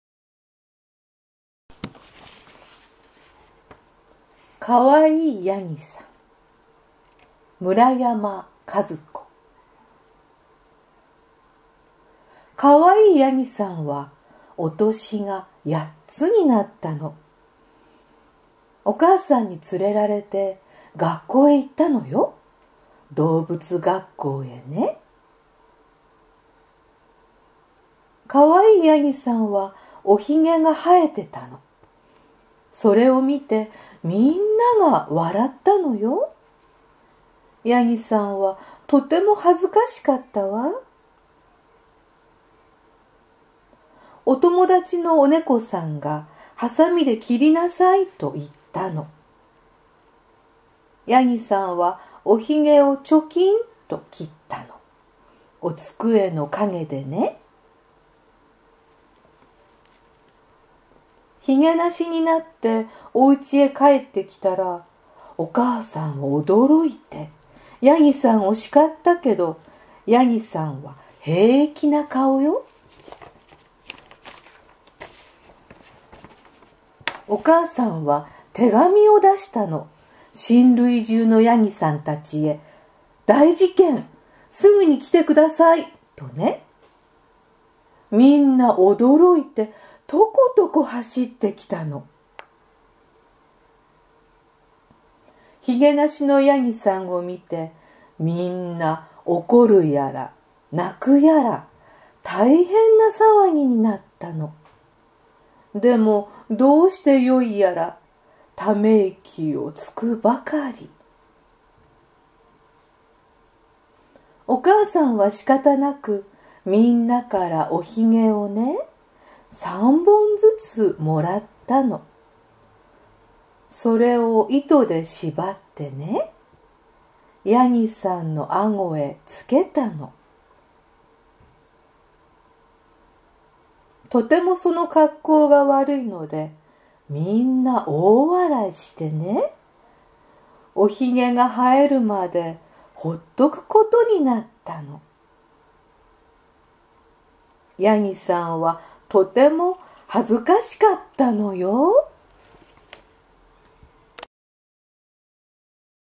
（童話）かわいいやぎさん 村山籌子 朗読：向井智子
青空文庫の中の童話を朗読させていただきました。
青空文庫の方々に　心から感謝いたします。

村山籌子さんについて。
1903－1946　昭和時代の児童文学作家。
明治36年11月7日生まれ。羽仁もと子にみとめられ,「子供之友」に寄稿。動物や野菜などを主人公に,ユーモアと機知あふれる幼年童話を創作,夫の村山知義(ともよし)が挿絵をえがいた。昭和21年8月4日44歳で死去。同年8月末に短編集「きりぎりすのかひもの」が刊行された。香川県出身。自由学園卒。